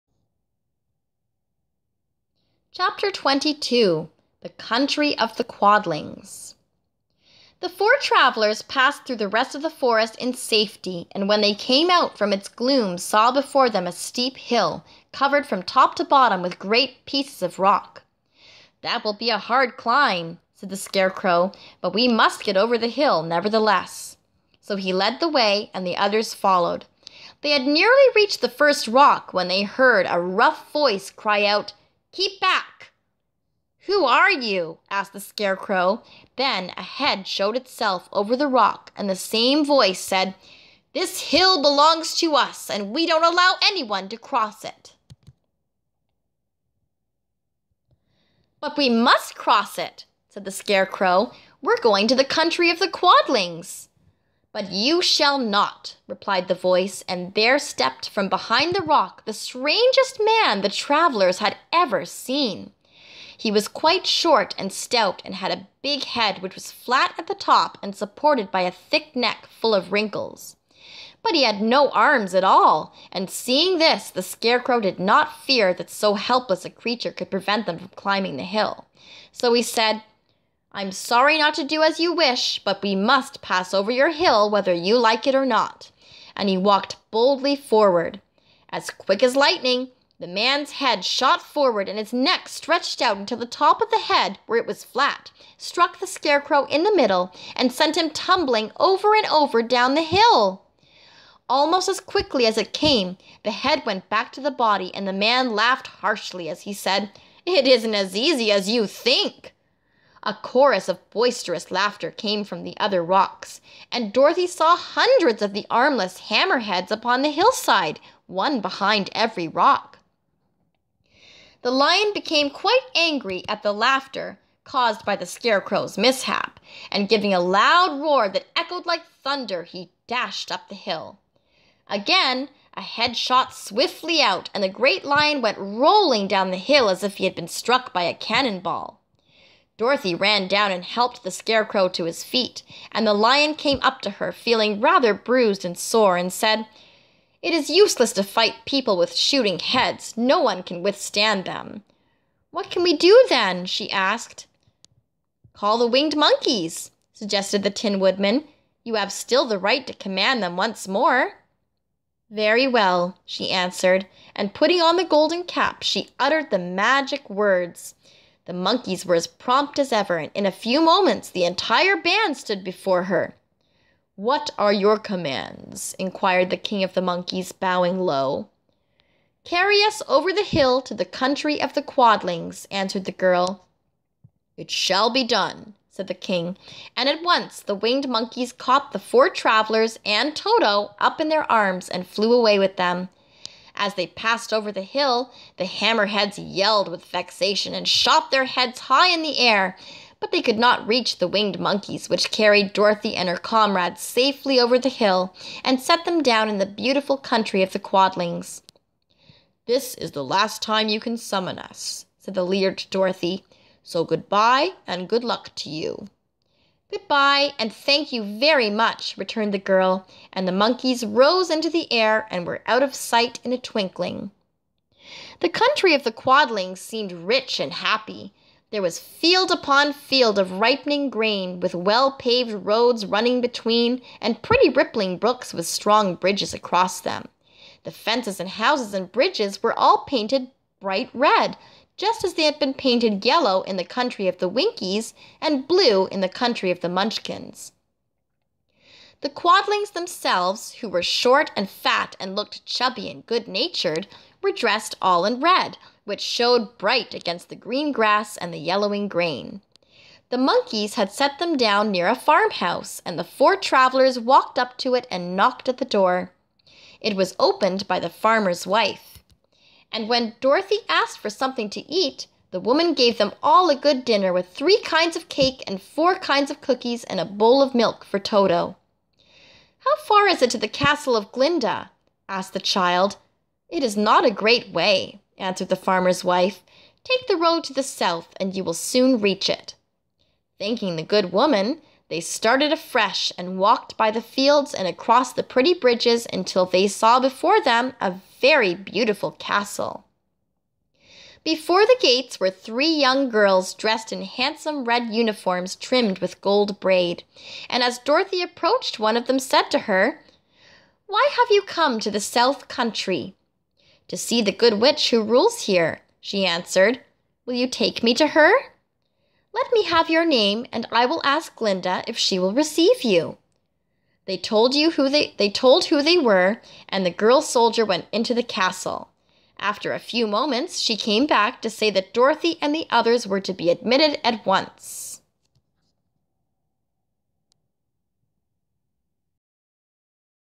Chapter Twenty-Two - The Country of the Quadlings - The Wonderful Wizard of Oz
Katie reading chapter twenty-two of The Wonderful Wizard of Oz by Frank L. Baum.